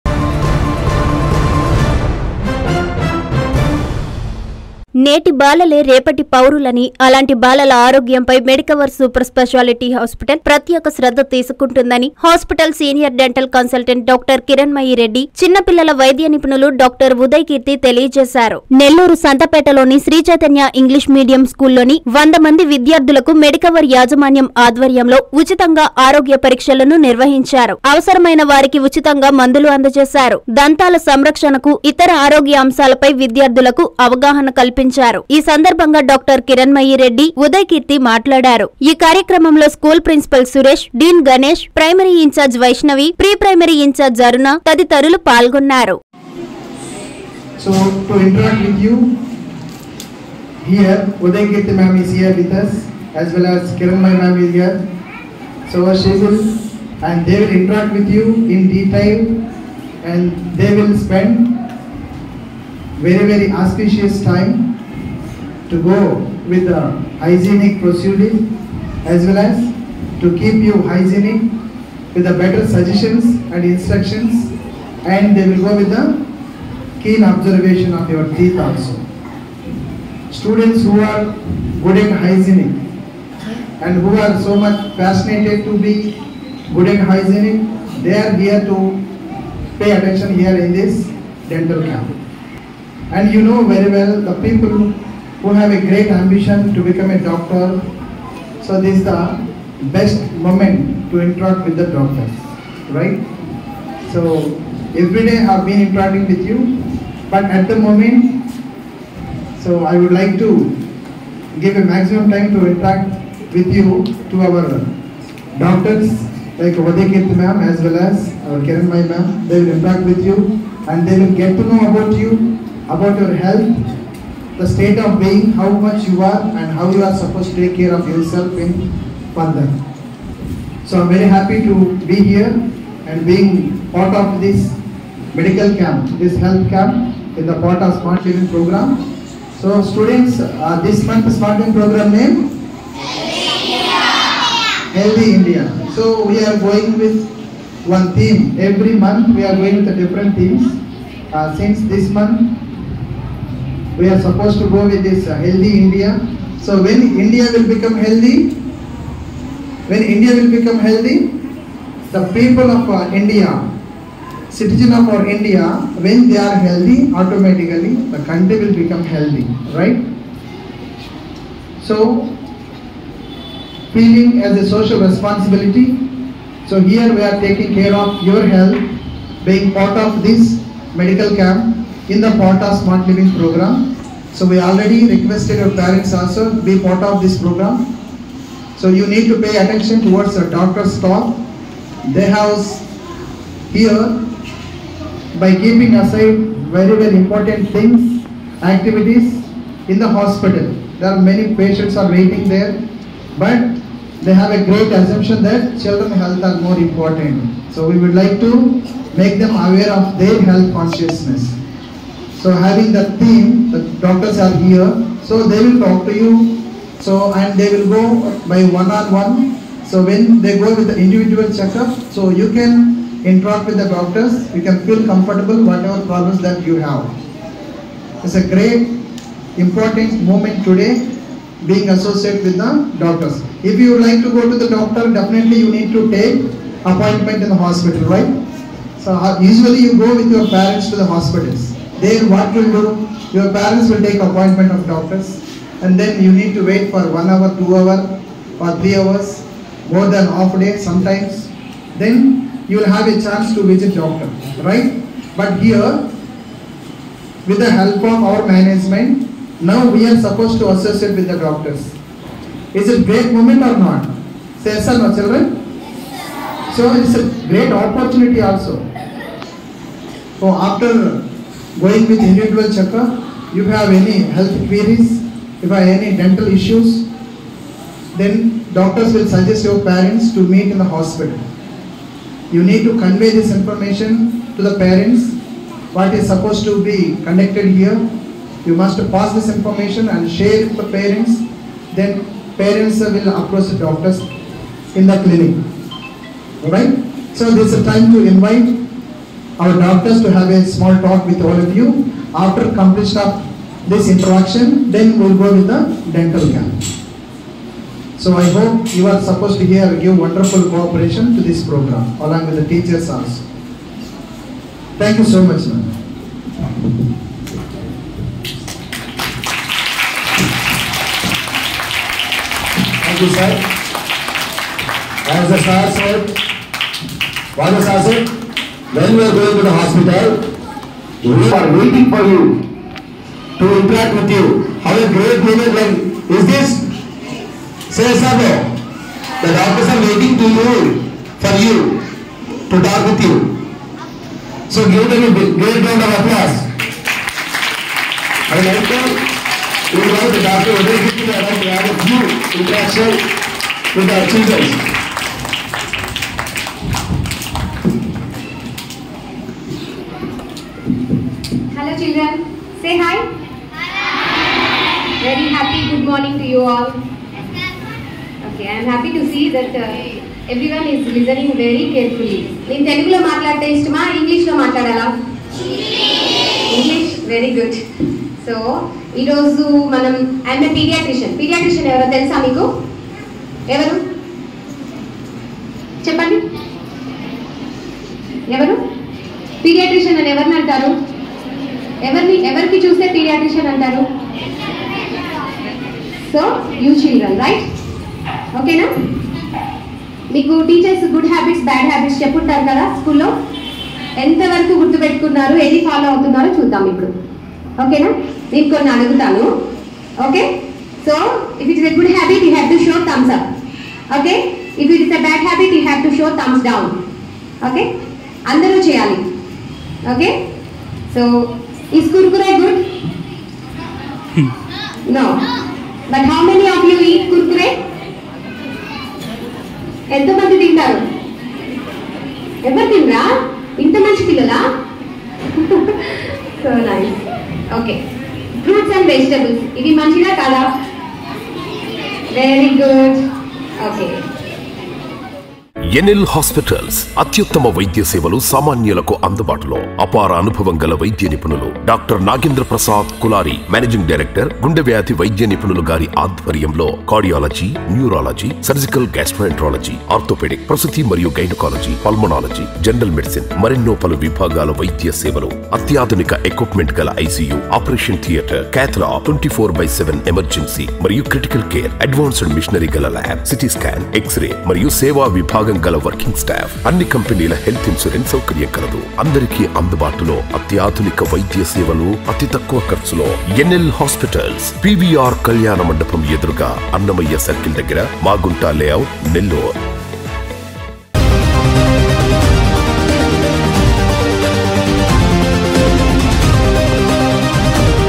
పిల్లల ఆరోగ్యంపై మెడికవర్ ప్రత్యేక శ్రద్ధ
- నెల్లూరు శ్రీ‌చైత‌న్య స్కూల్ విద్యార్ధులకు ఉచిత దంత పరీక్షలు
- విద్యార్థుల‌కు ప‌లు సూచ‌న‌లు చేసిన డాక్ట‌ర్ కిర‌ణ్మ‌యిరెడ్డి, డాక్ట‌ర్ ఉద‌య్‌కీర్తి